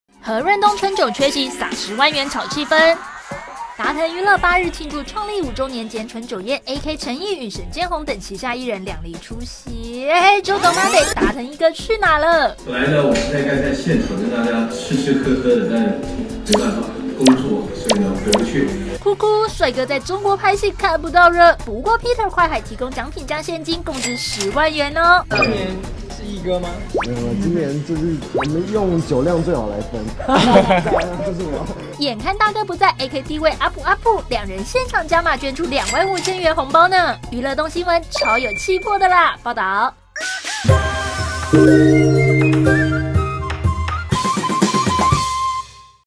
AK篡位一哥何潤東 過路財神代撒10萬紅包 | 台灣蘋果日報
達騰娛樂昨舉行春酒宴，也慶祝公司創立5周年，旗下藝人陳奕、沈建宏等帥氣出席，雖「達騰一哥」何潤東在中國拍戲缺席，但他誠意十足扮演財神爺錄製影片跨海撒錢，獎品加現金共值10萬元，人不到禮到炒氣氛。